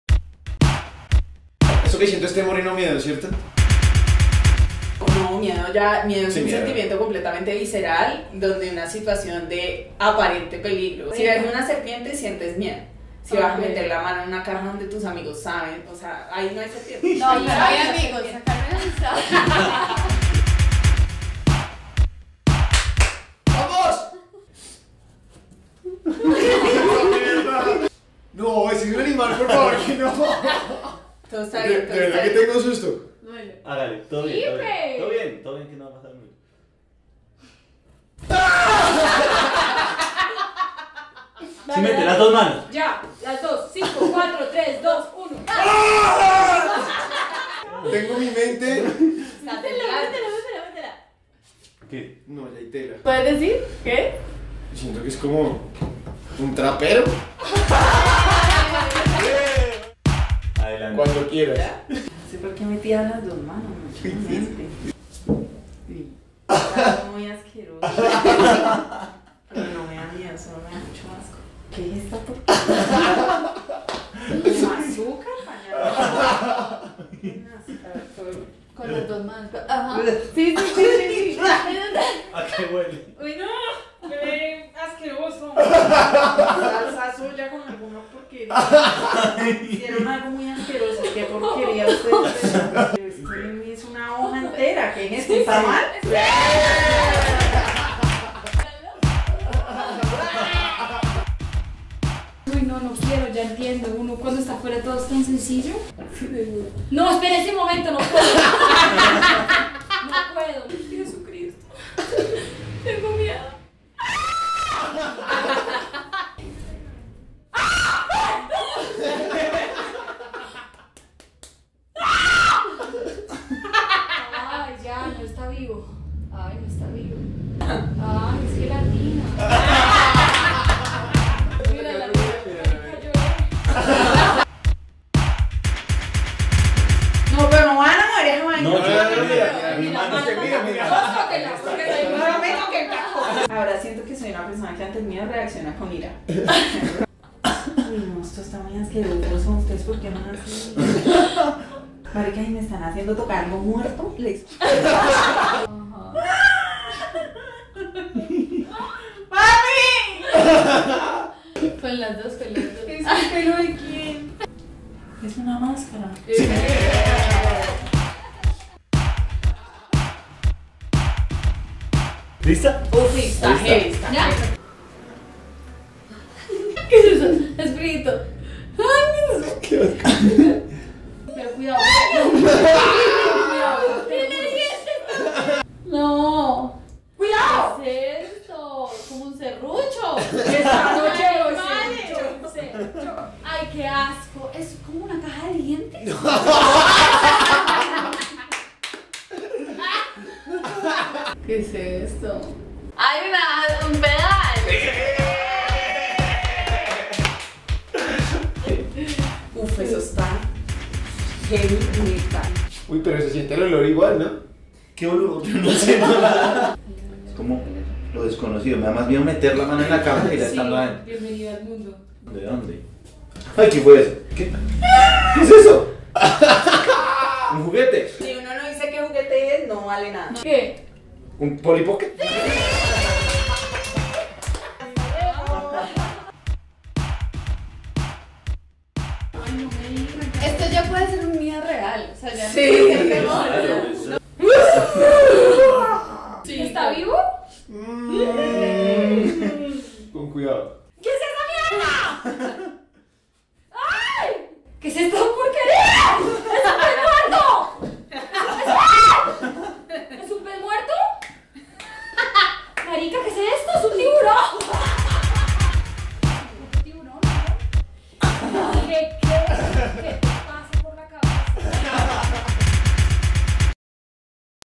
IMPROCHALLENGE con Amalia Andrade
En Bogotá estamos en Feria del Libro y por eso tenemos un capítulo especial de Improchallenge con Amalia Andrade! Nos retamos, jugamos y nos reímos de nuestros propios miedos!